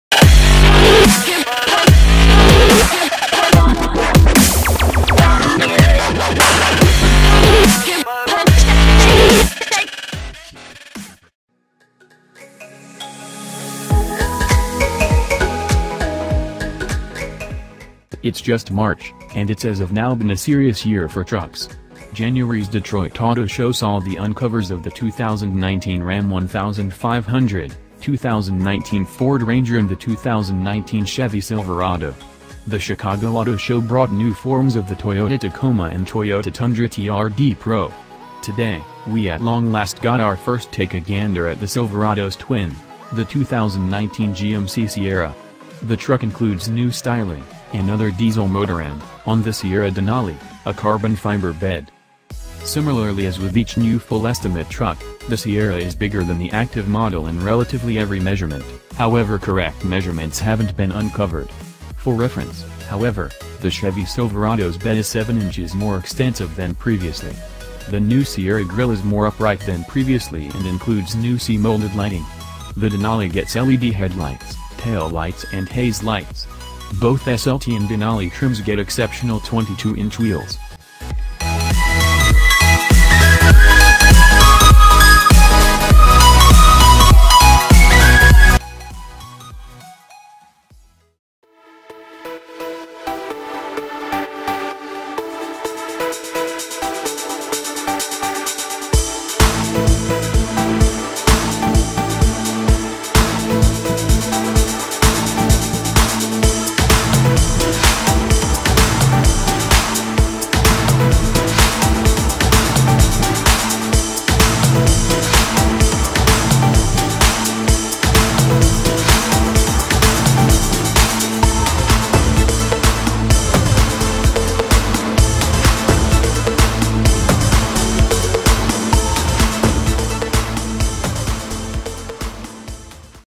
Watch This !!! 2019 GMC Sierra Denali HD Release Date Pictures
As with every new full-size truck, the Sierra is larger than the outgoing model in almost every dimension, though exact dimensions haven't been revealed. For reference, though, the Chevy Silverado's bed is 7-inches wider than before. The new Sierra grille is more upright than before and features new C-shaped lighting. The Denali gets LED headlights, tail lights and fog lights. Both SLT and Denali trims get unique 22-inch wheels.

The big improvements inside include a full-color heads-up display, trailer hitch assist apps and cameras, and the rear-camera mirror found in models like the Chevy Traverse and Cadillac CT6. Safety and convenience features include a surround-view camera, blind-spot monitoring, pedestrian detection and low-speed automatic braking.

As with the Silverado, the 2019 Sierra gets three engines, two gasoline V8s and a new diesel inline-6. The 5.3-liter V8 and 6.2-liter V8 will carry over, though with new stop/start technology and cylinder-deactivation that allows the truck to run on just a single cylinder. Yes, just one cylinder. Details on the 3.0-liter diesel haven't been announced, but, like the 6.2-liter V8, it will be paired with a 10-speed automatic. Denali models get a new "adaptive ride control," though what exactly that is (magnetic dampers?) was left out of the release.

tim4car fg

Music Free royalty By
Judul:dubstep